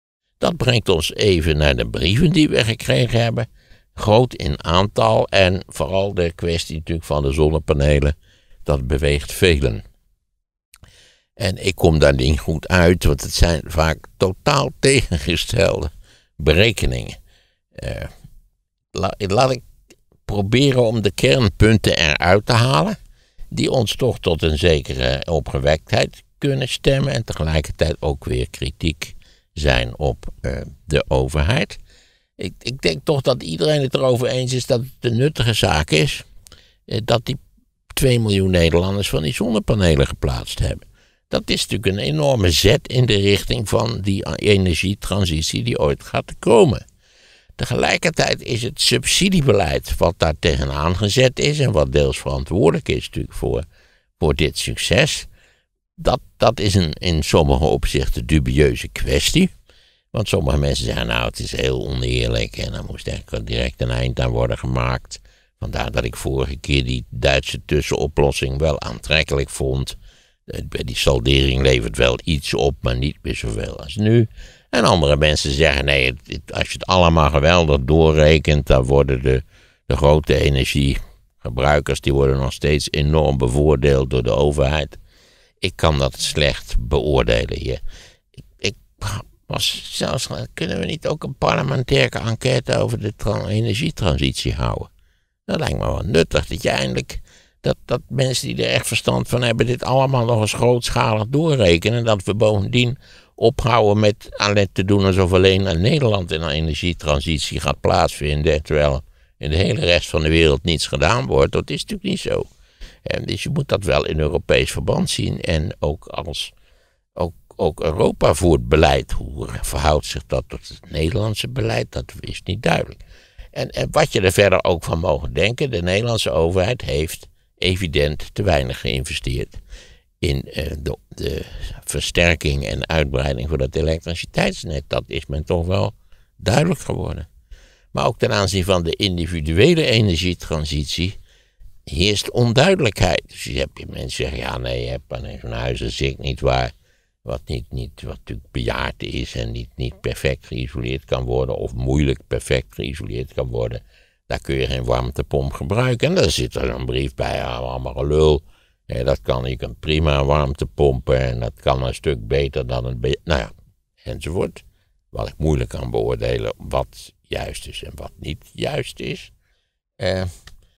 Maarten op de woke-tour: Schaft zonnepanelen aan
Maarten van Rossem heeft sindskort zonnepanelen. Daar is hij erg over te spreken.

▬ SHOWNOTES  ▬▬▬▬▬▬▬▬▬▬

▬ MEER MAARTEN  ▬▬▬▬▬▬▬▬▬▬

▬ LUISTERBOEKEN  ▬▬▬▬▬▬▬▬▬▬

▬ SOCIALS  ▬▬▬▬▬▬▬▬▬▬